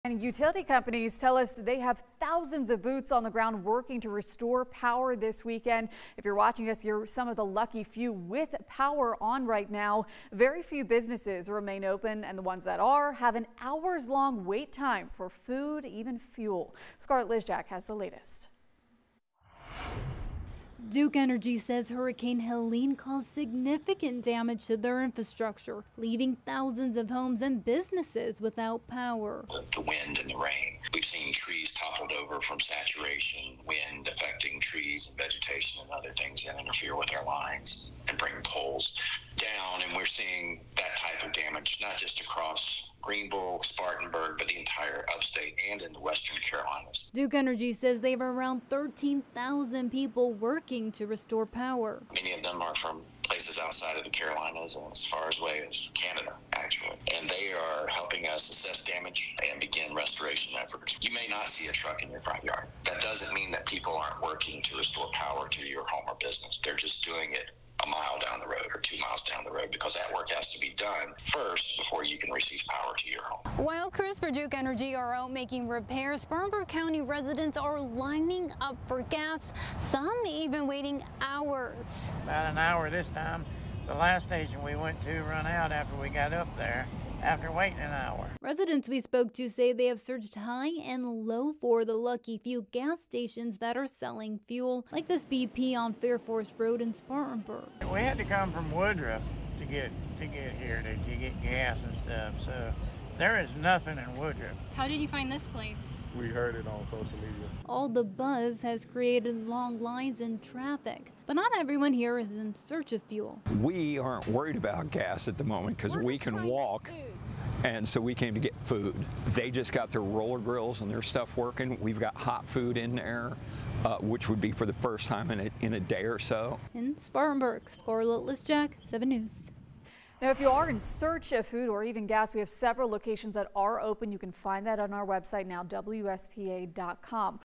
Long lines for food and fuel as power companies work to restore power
SPARTANBURG, S.C. (WSPA) - Utility companies said they have thousands of boots on the ground working to restore power this weekend. The few businesses open have hour-long wait times for food and fuel.